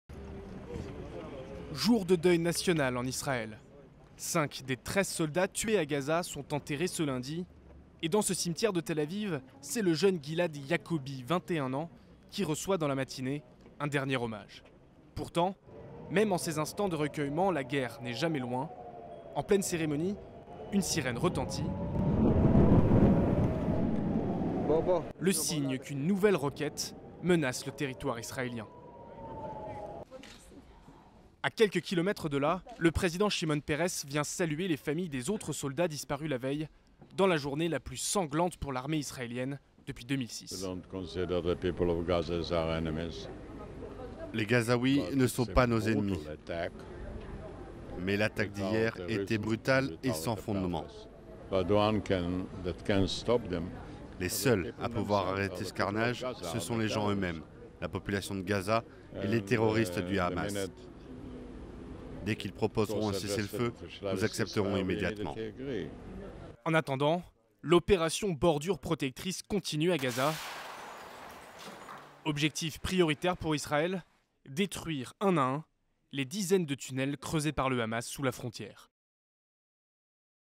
Israël rend hommage à ses soldats tués
Du côté israélien aussi, on pleure aussi ses disparus. Alors que sept nouveaux soldats de Tsahal ont été tués à Gaza ce lundi après-midi, portant à 25 le nombre de militaires tués depuis le début de l'offensive, les funérailles des cinq premiers militaires morts au combat ont eu lieu sur le mont Herzl, à Jérusalem. Un deuil qui n'entame pas la détermination de l'armée israélienne à poursuivre l'opération "Bordure protectrice" et la destruction des tunnels du Hamas.